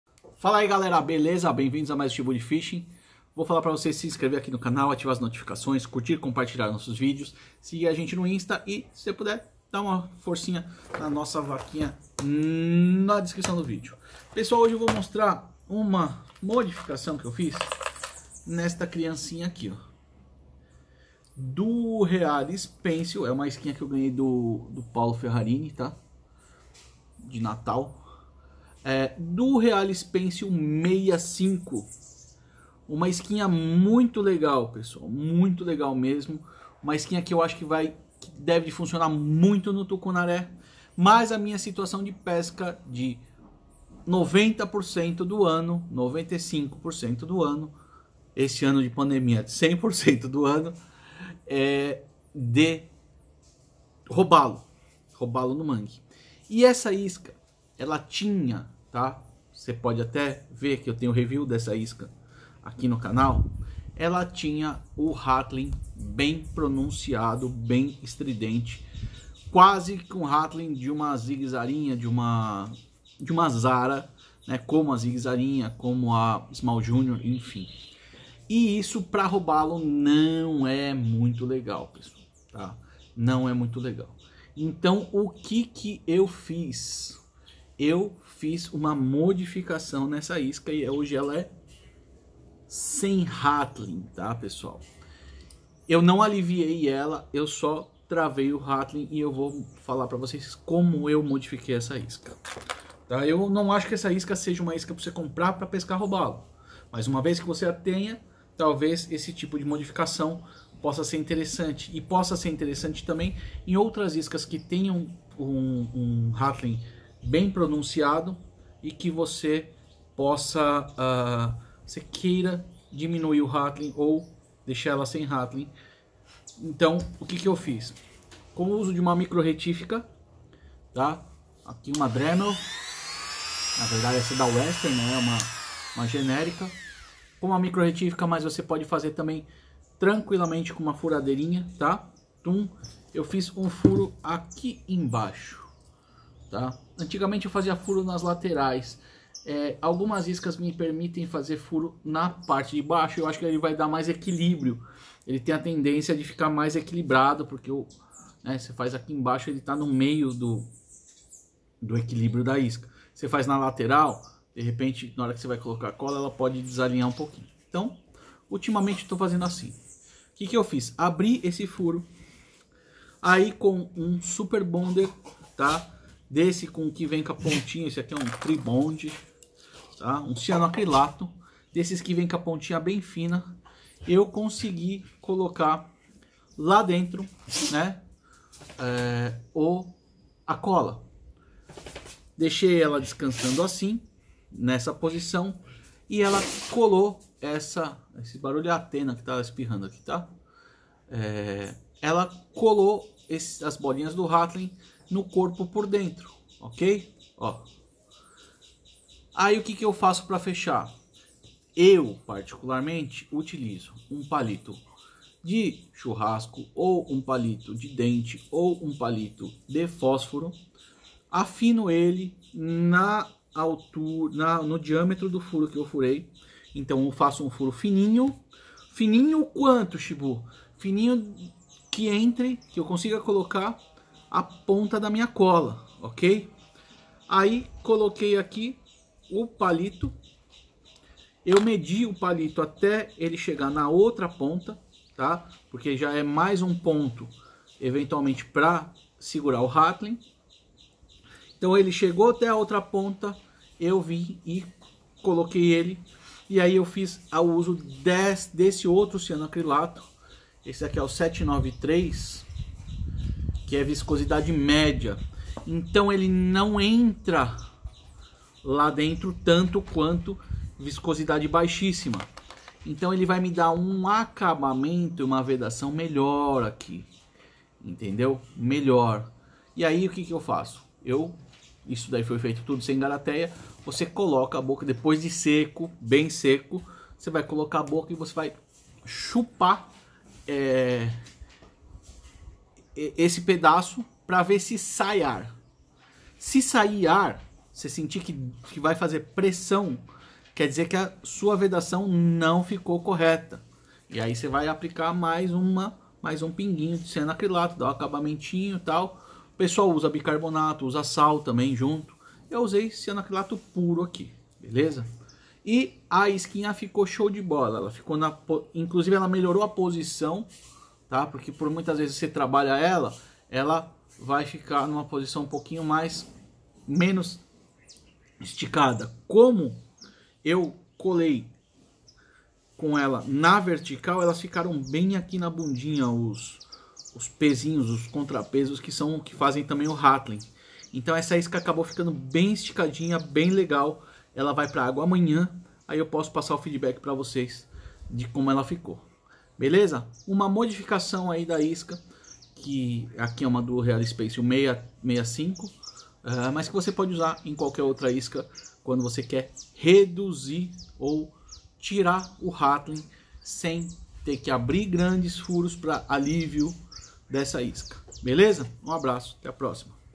Alteração da Duo Realis Pencil 65 para robalo
Neste video eu mostro uma alteração que eu fiz na duo reallis 65 para deixar do jeito que eu gosto para robalos